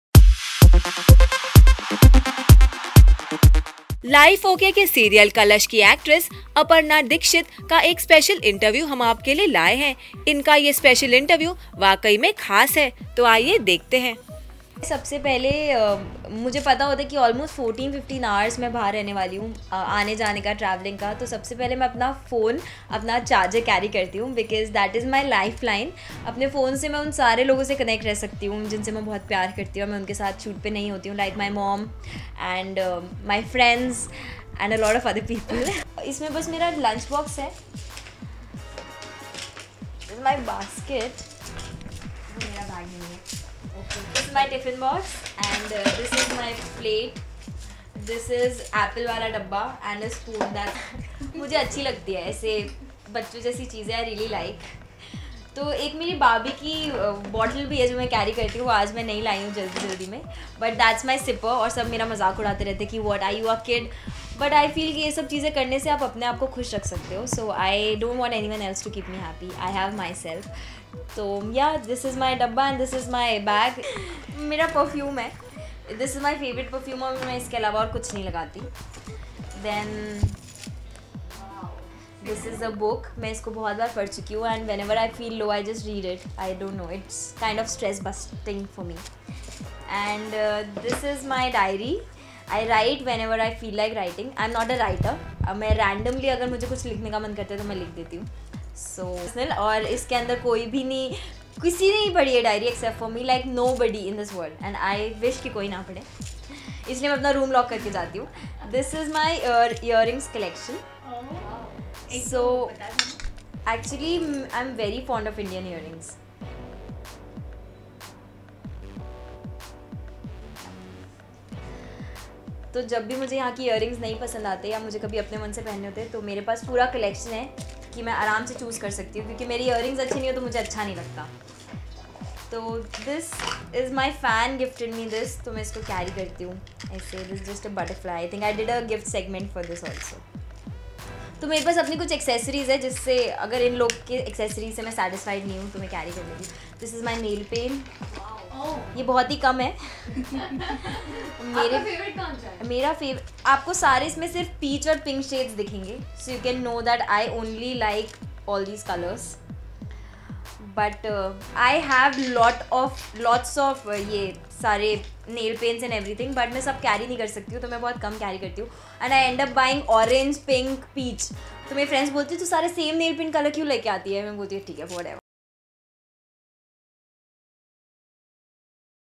Kalash Actress Aparna Dikshit Talks About Her Daily Routine
Kalash Actres  Aparna Dikshit Talks About Her Daily Routine

Life OK ke serial ‘Kalash’ ki actress Aparna Dixit ka ek special interview hum aapke liye laaye hai. Inka ye special interview, vakayi mei khas hai.. Toh dekhte hai.